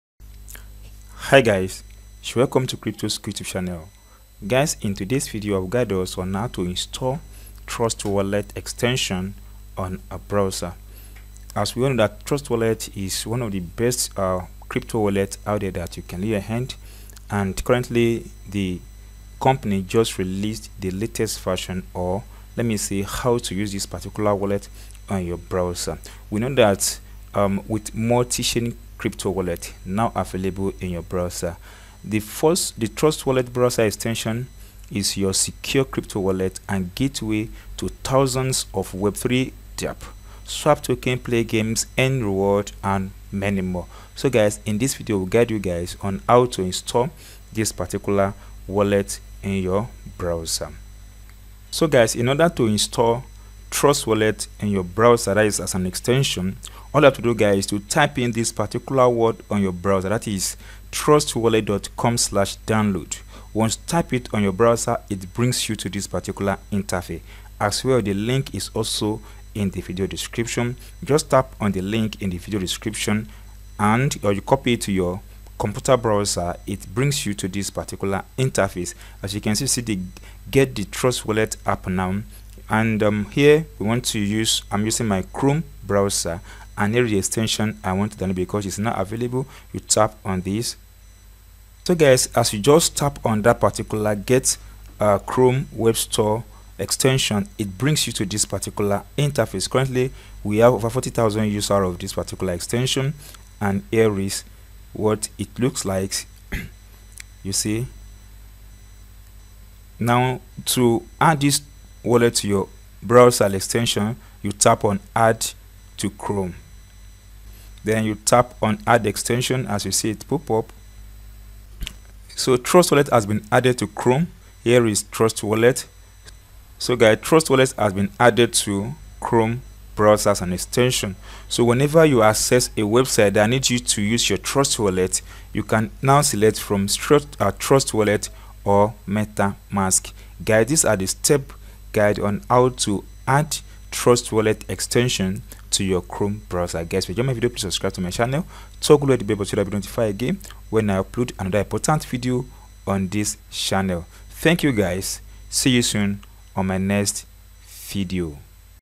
How to Install Trust Wallet Extension on Google Chrome Browser On Windows Computer
In this video, you will learn simple and easy way to add Trust Wallet Extension to Google Chrome Browser